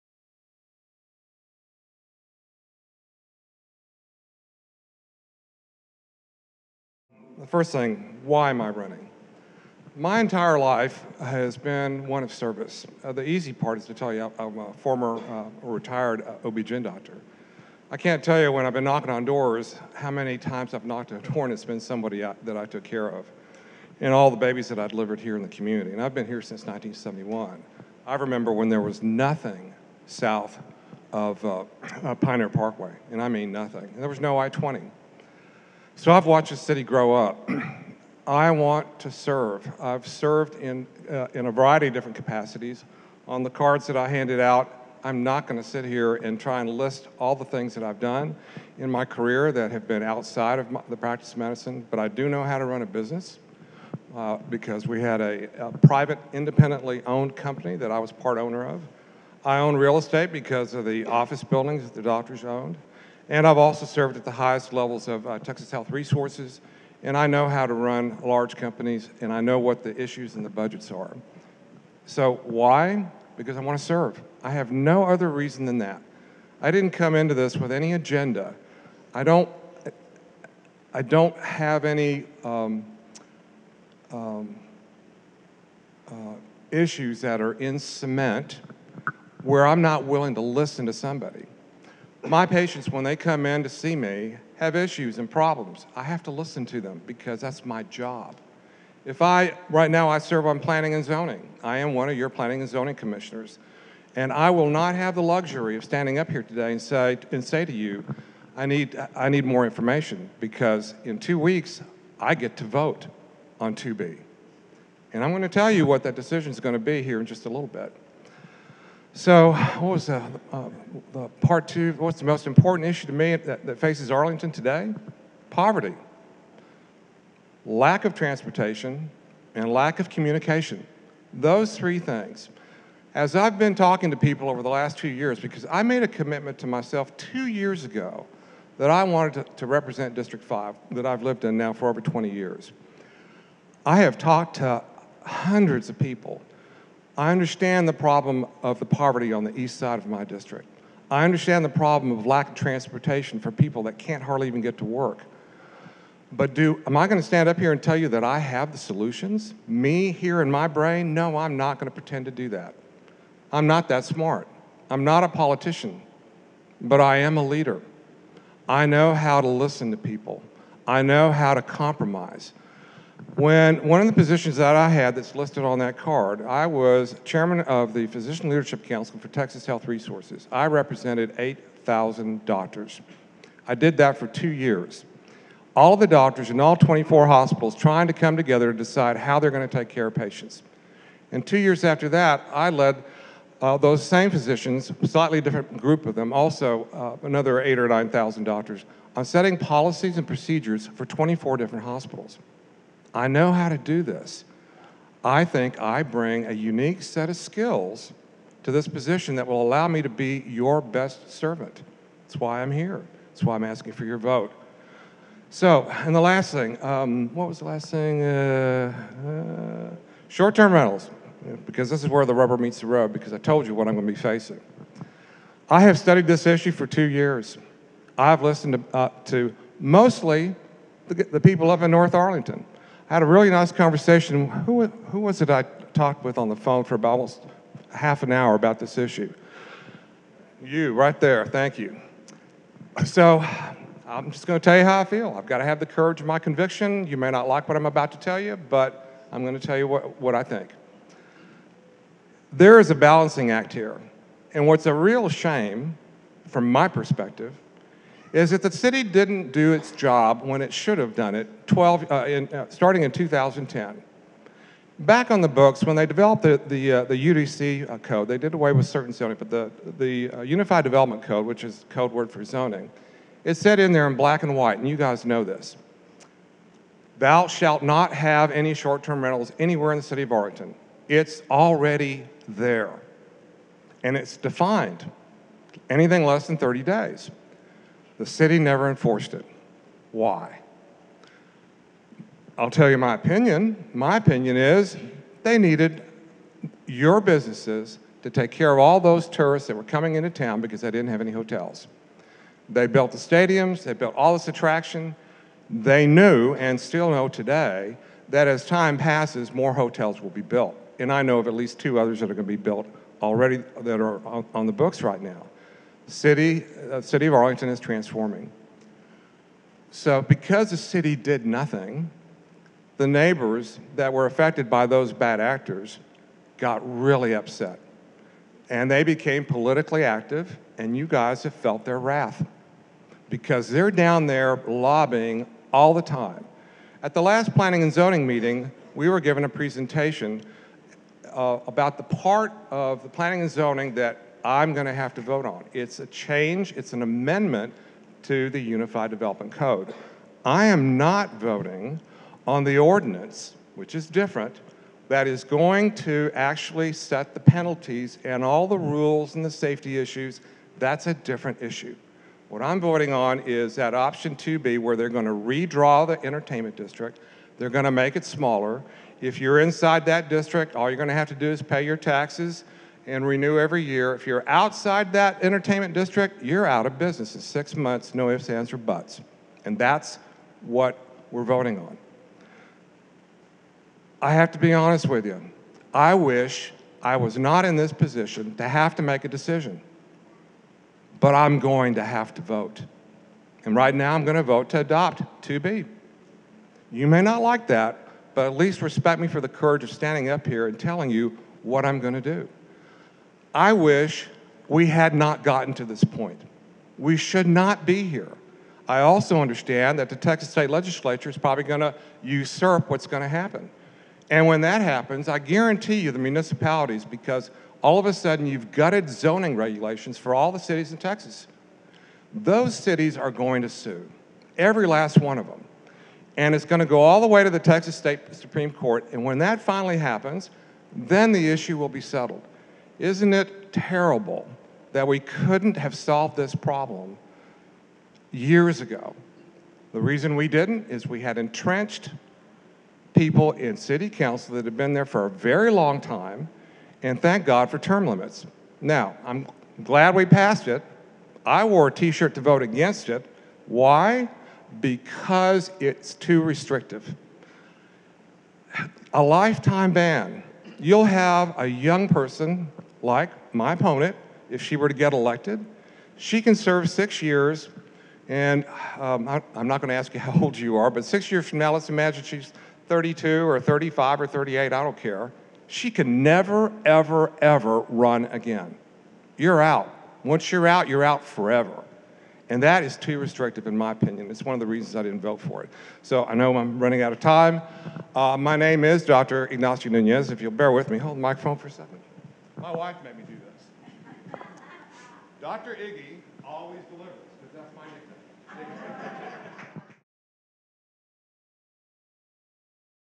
START General Meeting - Ignacio Nunez for Arlington District 5
Short Term Accommodations for Residents & Tourism (START) - General Meeting & BBQ regarding Short Term Rental was held on March 9th, 2019. All Arlington City Council and Mayoral Candidates were invited, and 13 out of 17 total were able to attend. They all shared their views on the same 4 questions:

1) Why are you running for office?
2) What is the most important issue(s) facing Arlington?
3) How are you going to solve that issue(s)?
4) If elected would you support any ordinance that would put out of business any short term rentals operating in a responsible manner?